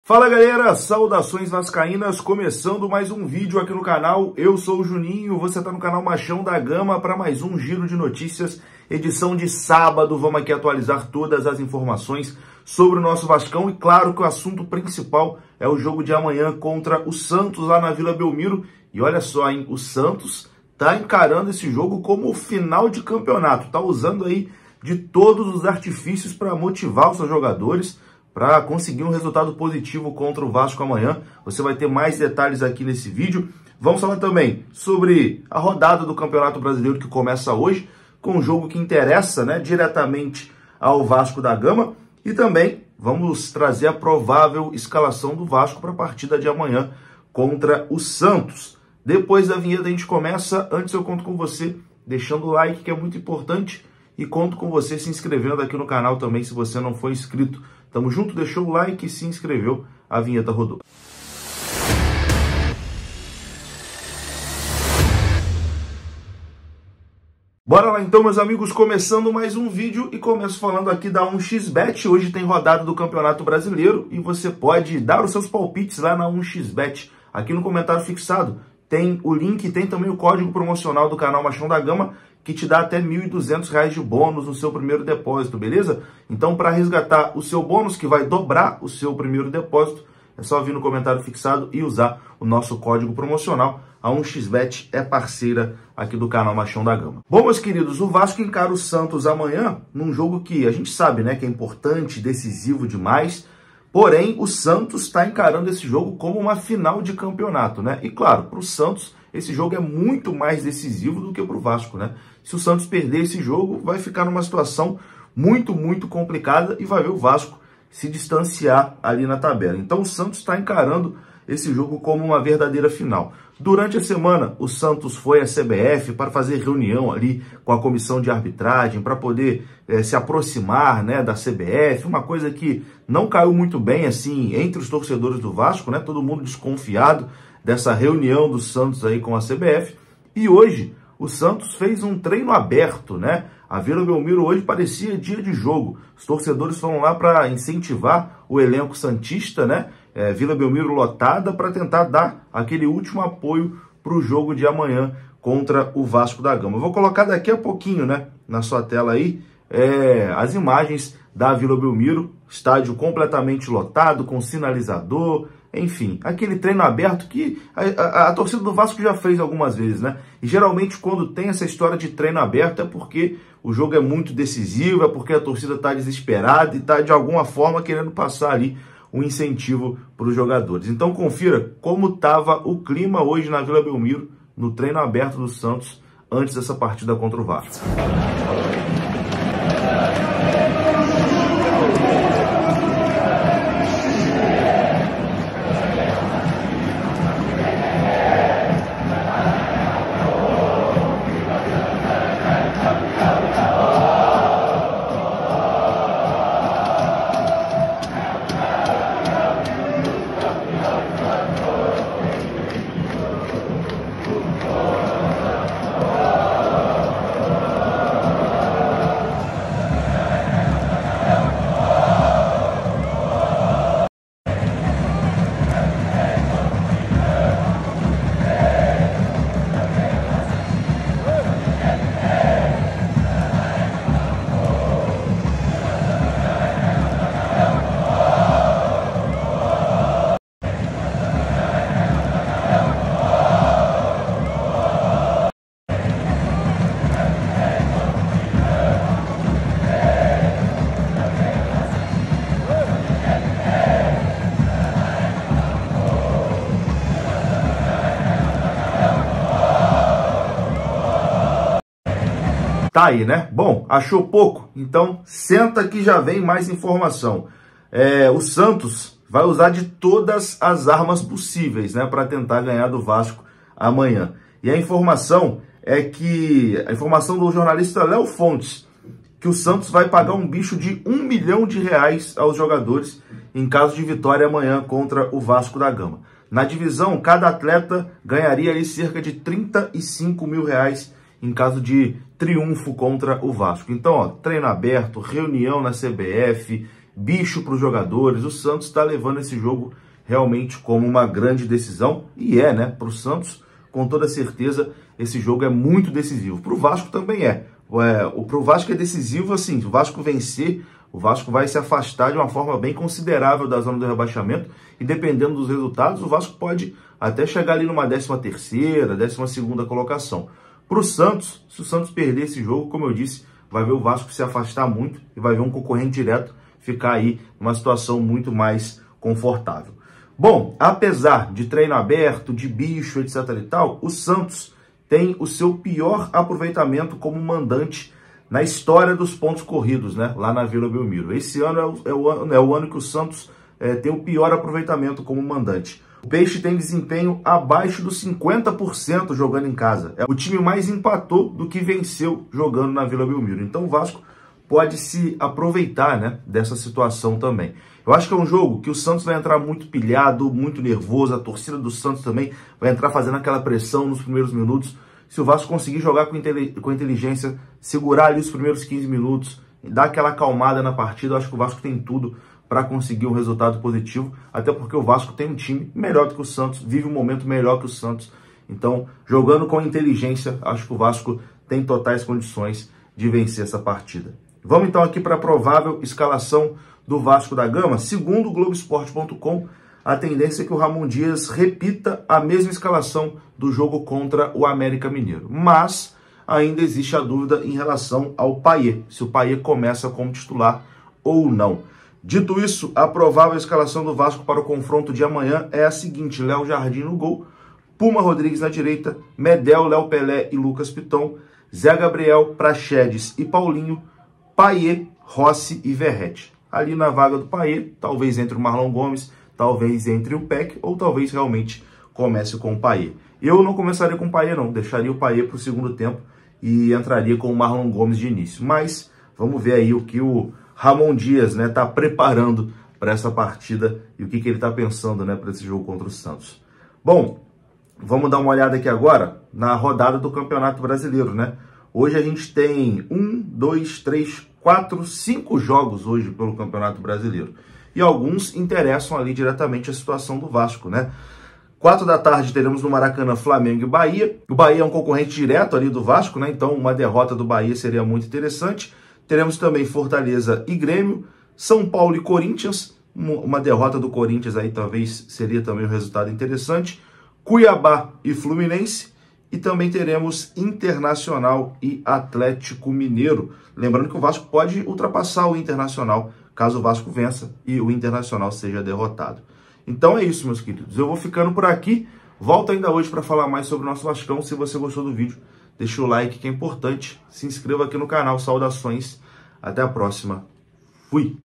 CLIMA DE FINAL: SANTOS PROMETE BICHO MILIONÁRIO PARA GANHAR DO VASCO | PAYET TITULAR OU RESERVA?
CUPOM: VASCODAGAMA
Giro de Notícias com as últimas informações do Vasco. Confira!